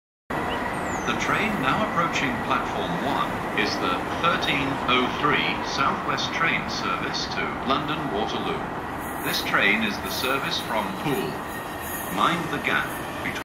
Phil Sayer South West Trains Announcemont
Accidently recorded a Phil Sayer announcement, so here it is